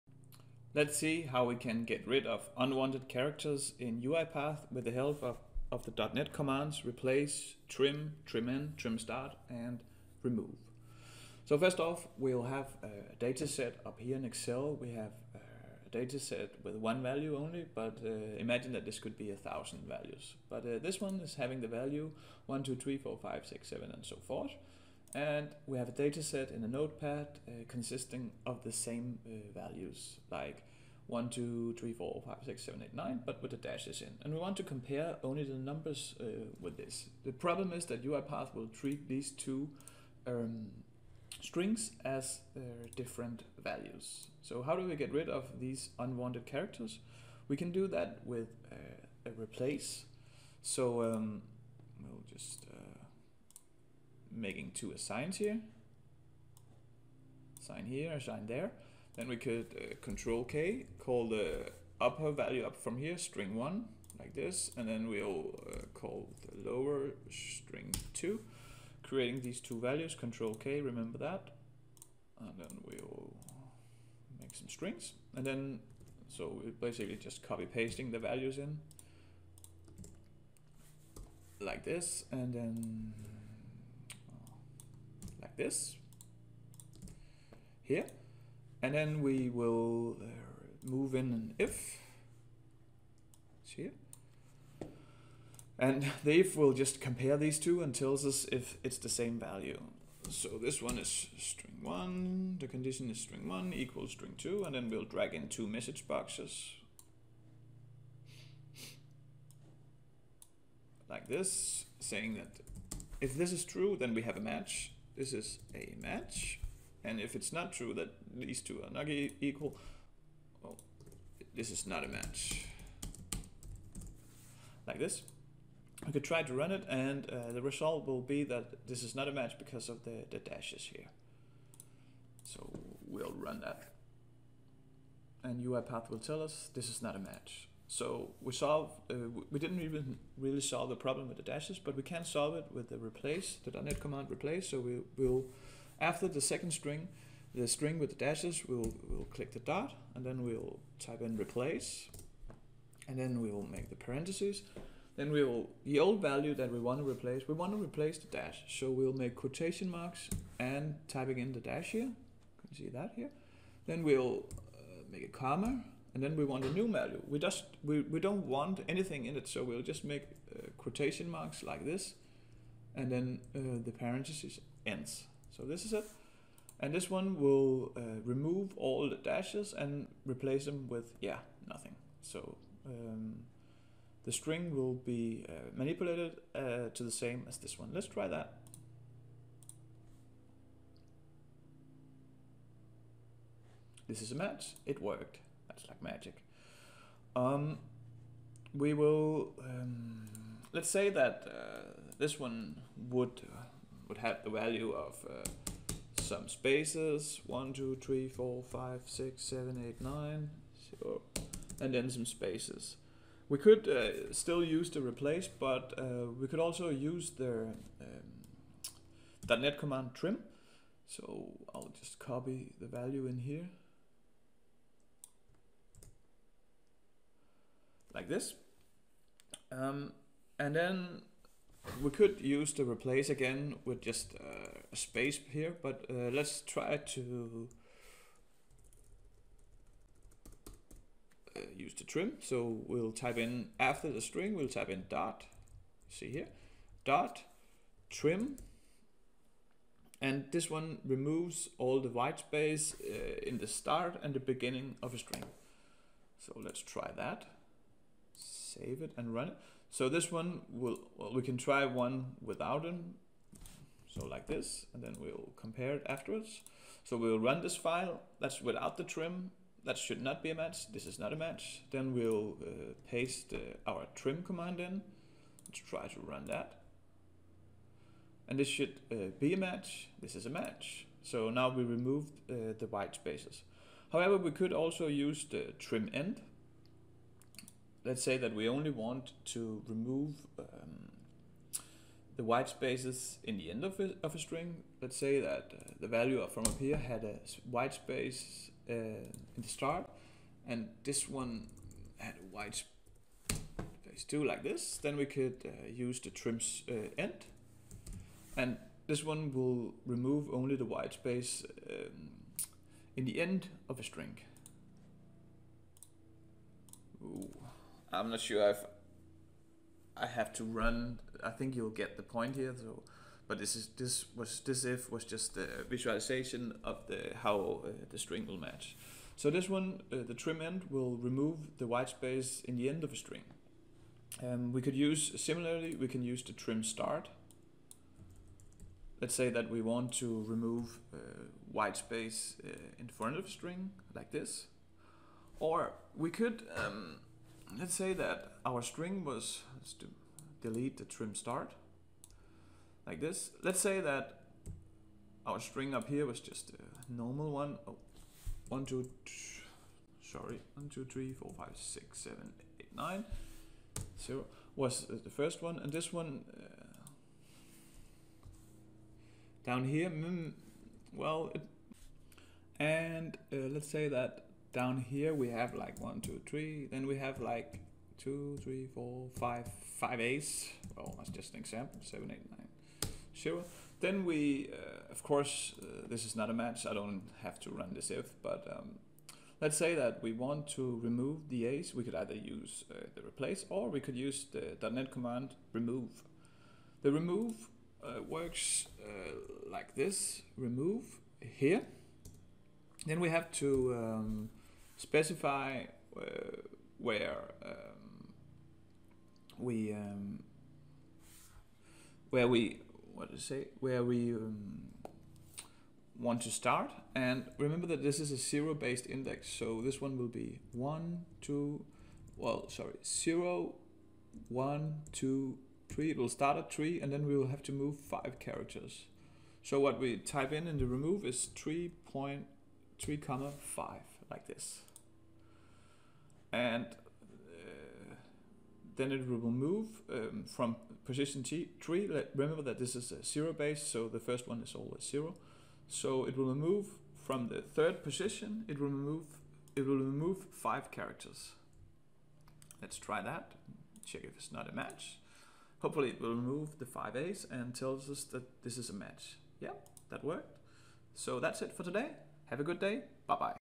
UiPath: Replace, Trim, TrimStart, TrimEnd and Remove (.Net)
In this tutorial it's described how we can remove unwanted characters with the help of the .net commands Replace, Trim, TrimStart, TrimEnd and Remove.